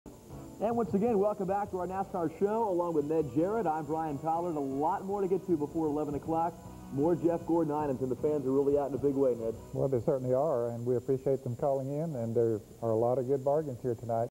Brian Collard Interviews Ned Jarrett
Brian and Ned on the set of the live in studio HSN show.